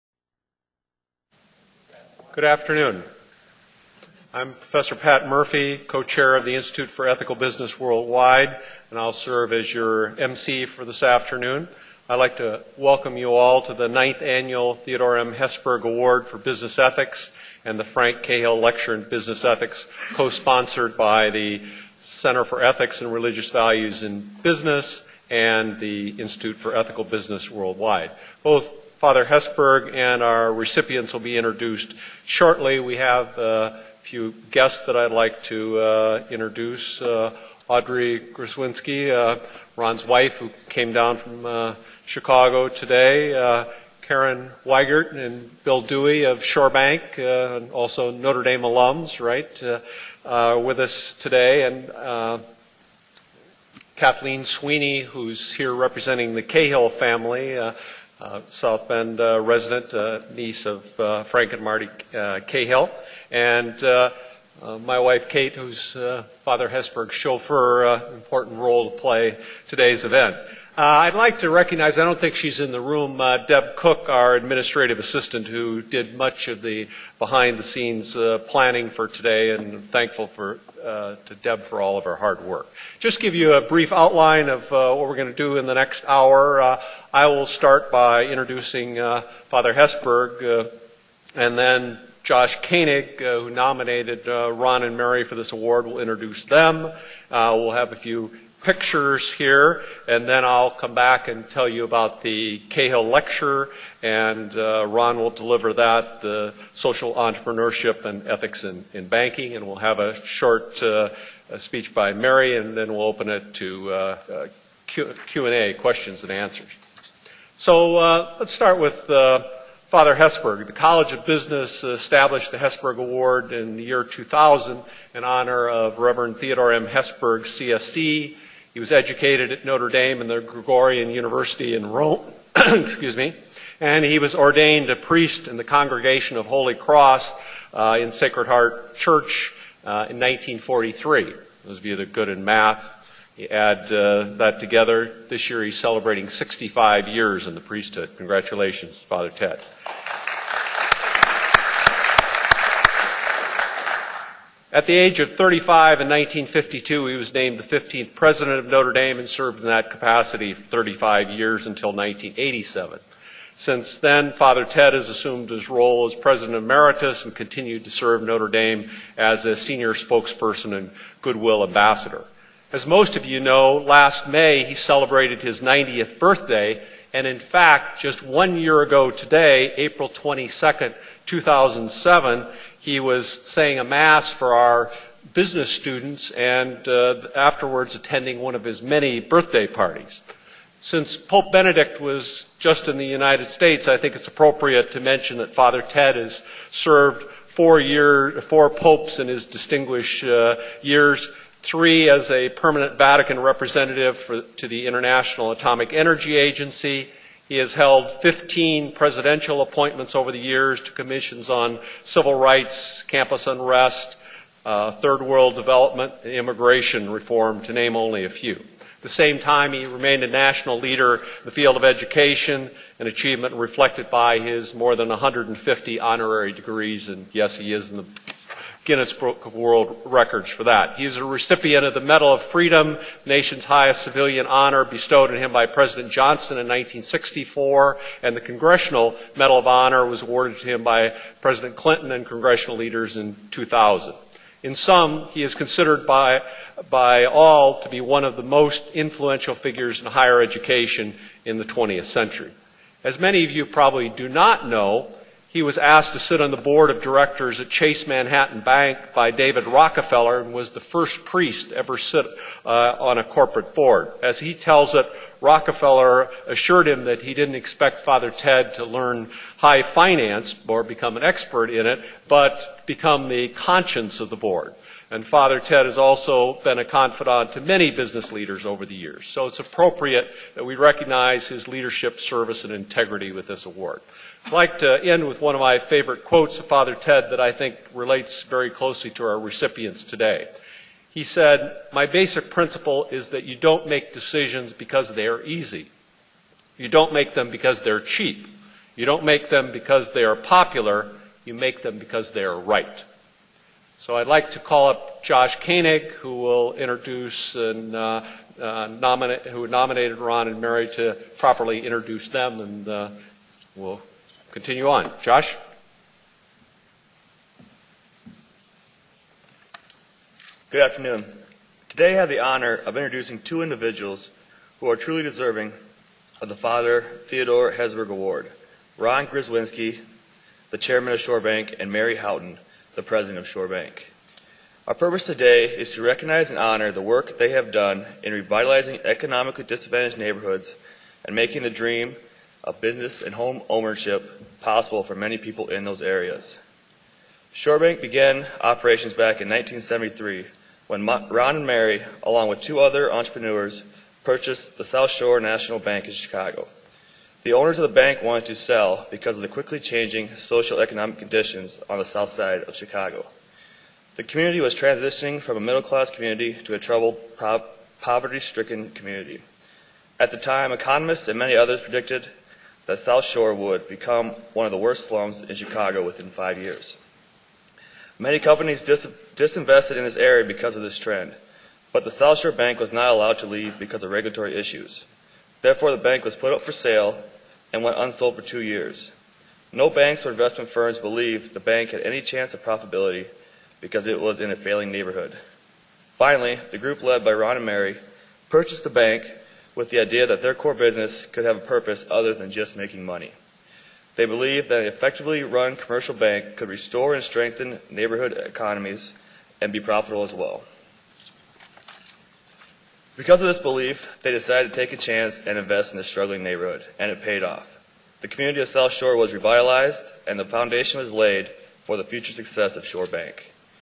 Ron Grzywinski and Mary Houghton speak at Notre Dame, PT 1
ShoreBank's Ron Grzywinski and Mary Houghton speak about community development and environmental banking upon receiving the 2008 Hesburgh Business Ethics Award from Notre Dame University.